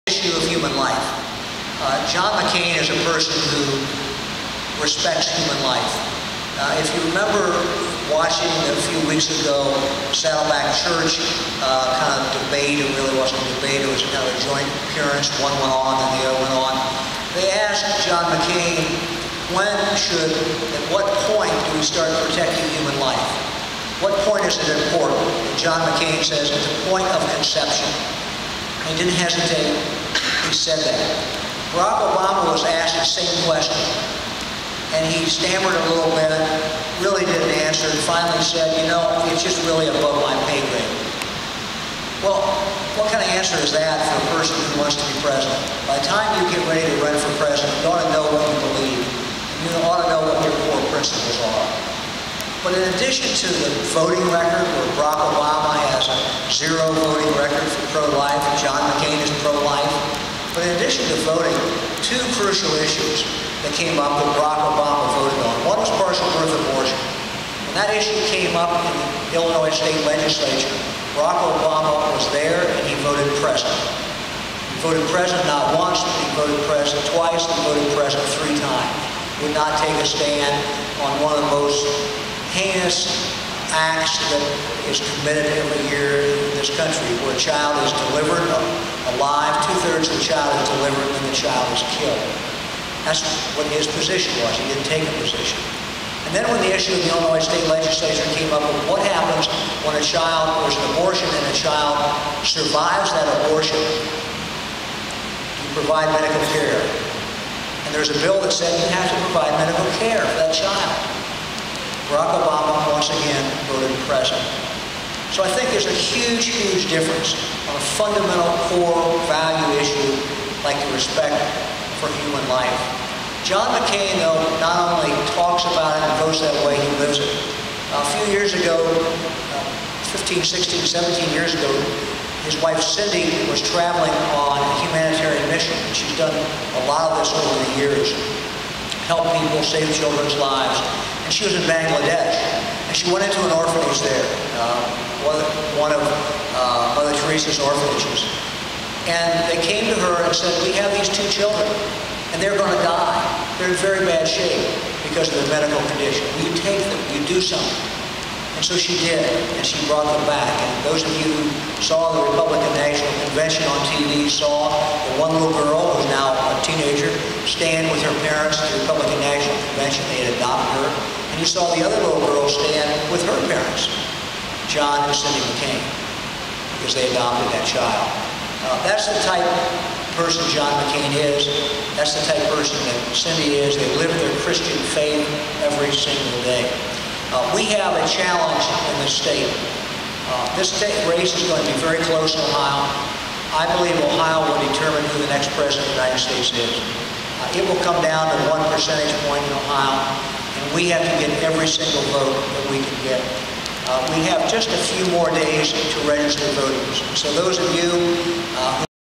Rally for McCain-Palin at OCU Part 3
Mike Dewine gives the TKO to Obama while stumpin for McCain!